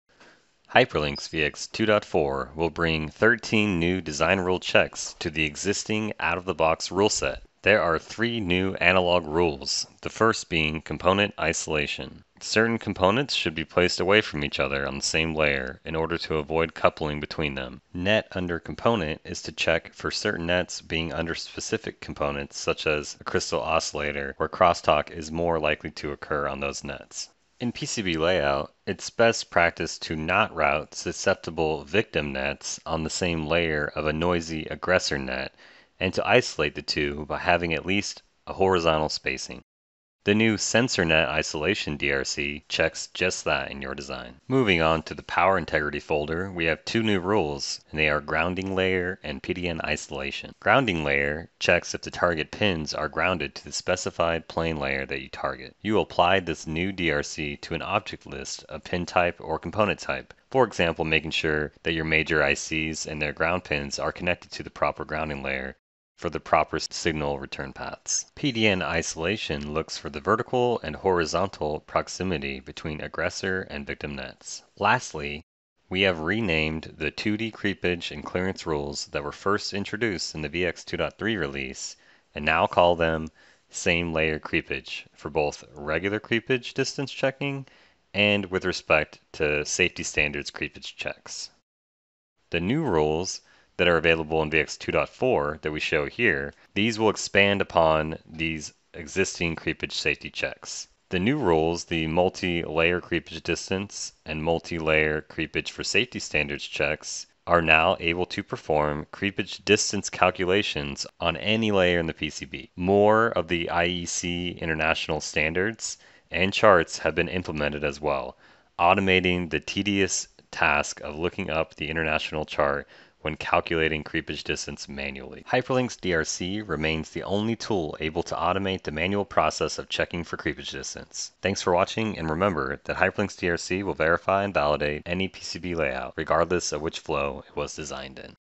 New Electrical Design Rule Checks
HyperLynx VX.2.4 brings many new electrical design rules, including analog rules for net under component, component isolation, and censor net isolation. New power integrity rules include PDN isolation and grounding layer. Creepage rules were added and enhanced to perform distance calculations on any layer of the PCB. To save research time, additional IEC international standards and charts were also integrated into the tool.